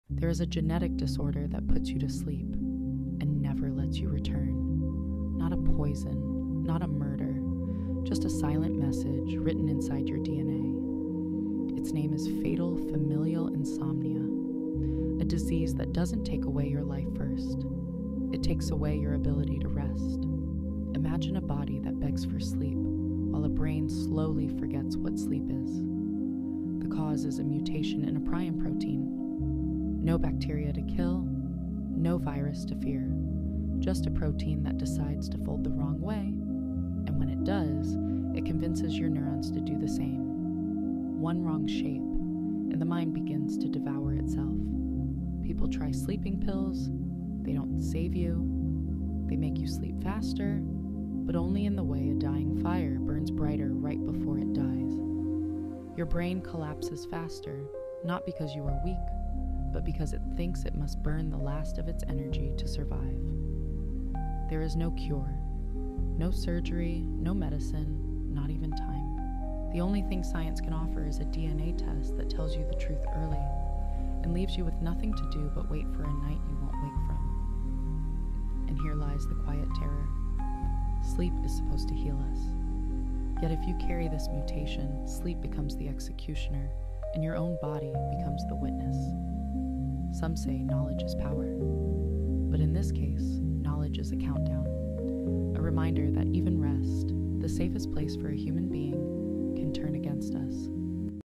Fatal Familial Insomnia — The Disease That Erases Sleep
There is a disorder that doesn’t kill you quickly…
It kills you slowly, by taking away the ability to ever sleep again.

Fatal Familial Insomnia is a genetic condition caused not by infection, but by a protein misfolding inside your brain — convincing other proteins to collapse with it. It is one of the most haunting illnesses known to science, because there is no cure… only awareness.

Welcome to The Pandora Lab — where hidden reality is unlocked through science.